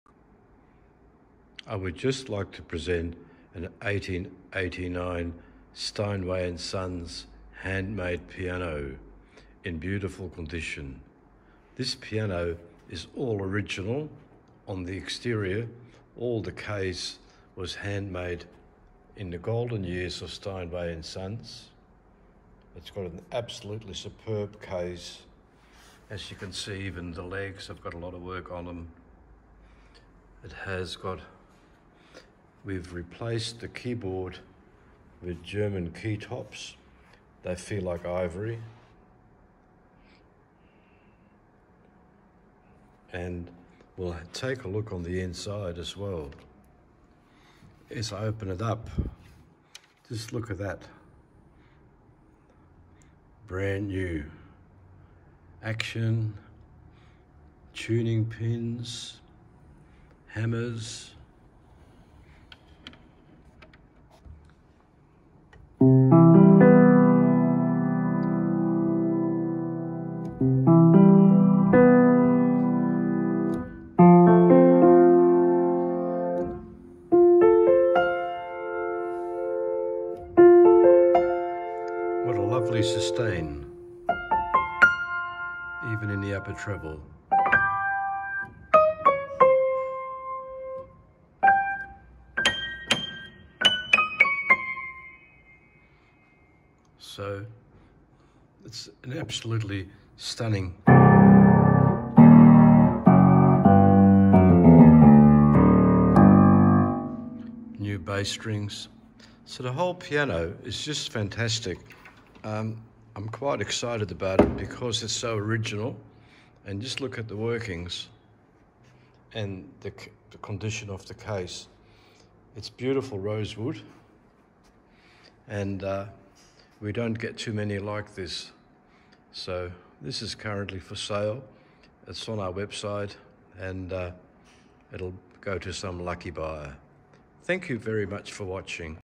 IMG 0073 Beautifull Steinway & Sons Piano, circa 1898 fully restored.
A brief look at this wonderfull instrument, made during the "Golden Years" at Steinway & Sons.